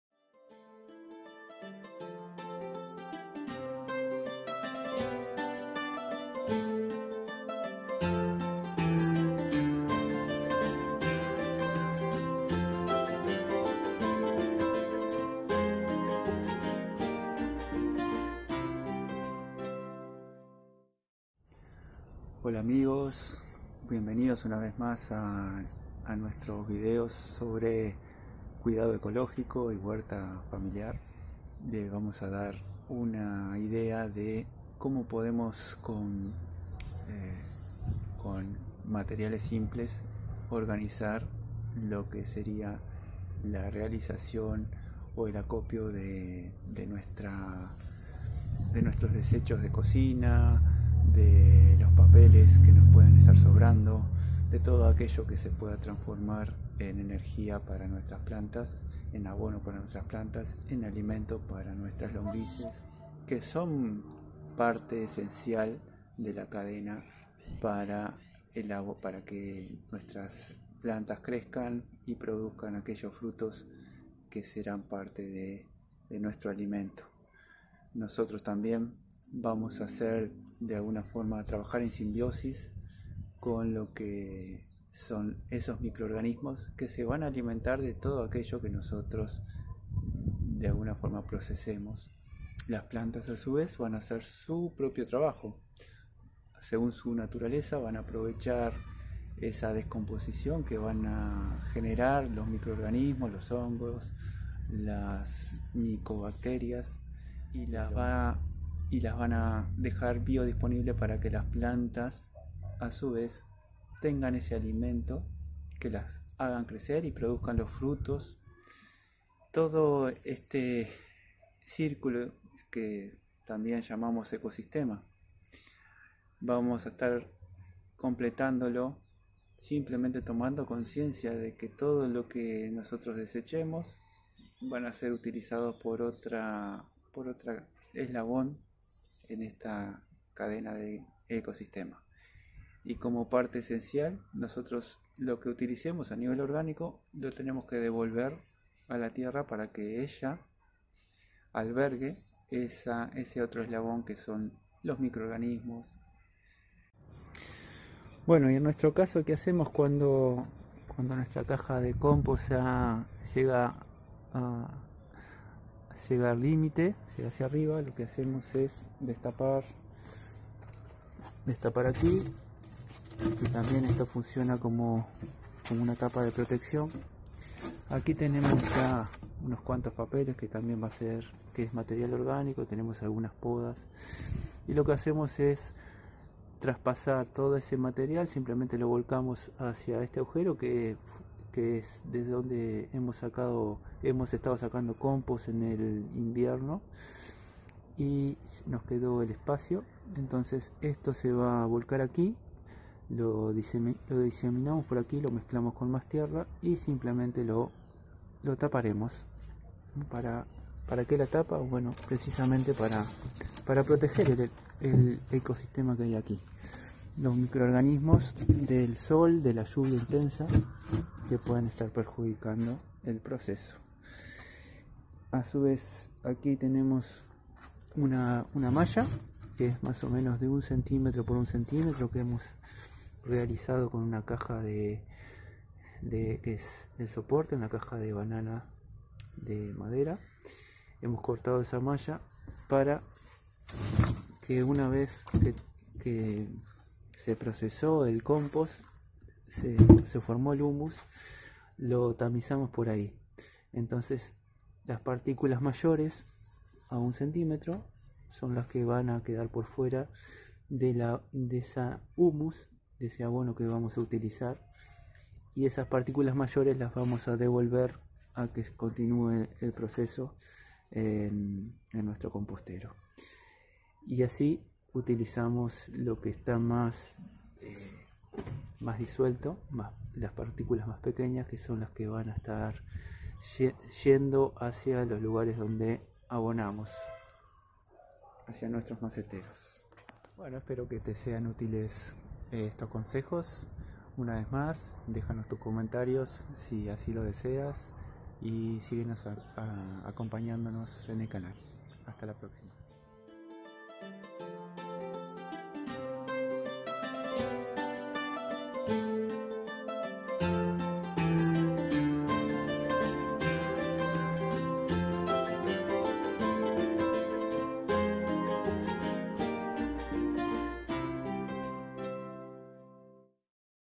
Ecosistema del compost - Huerta organica
En este video te contamos algo mas sobre el ecosistema del compost y te damos un ejemplo de como  procesarlo de forma practica en un pequeño espacio.

Anímate a iniciar el tuyo si aún no lo tienes y si ya lo has hecho coméntanos como lo haces.

Imágenes de Espacio Vida Sen y Pixabay